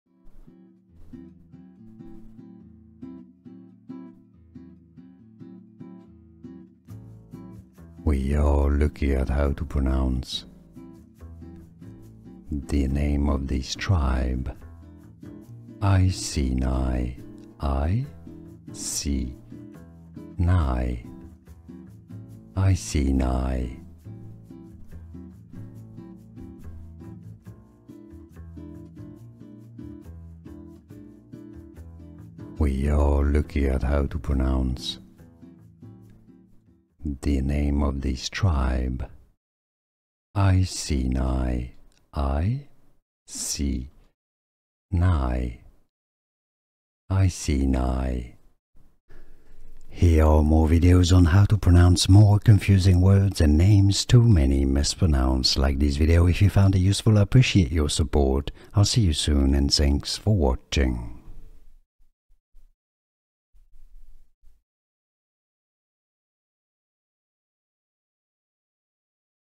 How to Pronounce Iceni Tribe
This video shows you How to Pronounce Iceni Tribe, pronunciation guide.

00:00 - Pronunciation Intro
00:16 - How to Pronounce Iceni Tribe
00:55 - More Difficult Pronunciations

How to Pronounce Iceni Tribe. What does this word/name mean? Words’ meaning, dictionary definition, explanation.